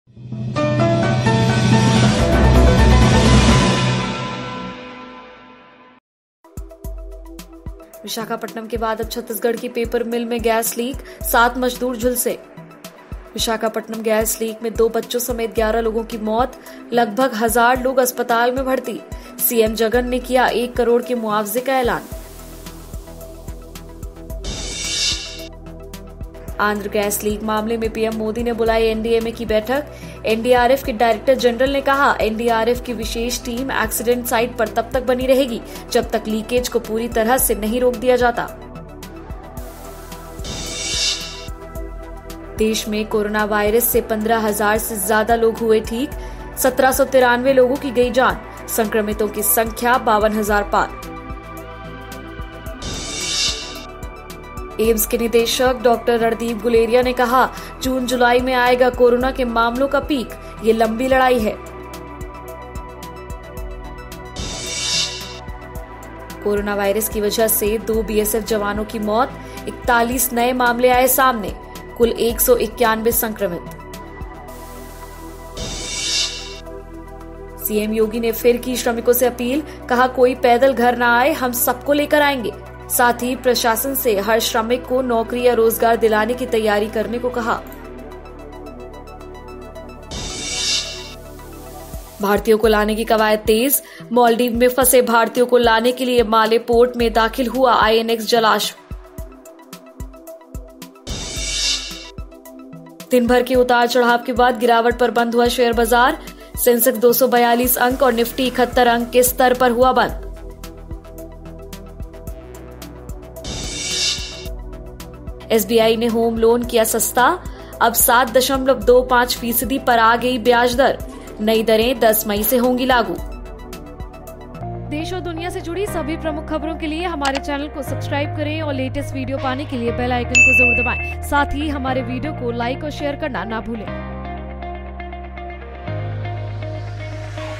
Visakhapatnam में फैली 'मौत' की गैस, दिखा भयावह मंजर | Vizag gas leak | HS News
विशाखापट्टनम गैस लीक में 2 बच्चों समेत 11 लोगों की मौत, लगभग 1000 लोग अस्पताल में भर्ती. सीएम जगन ने किया एक करोड़ के मुआवजे का एलान,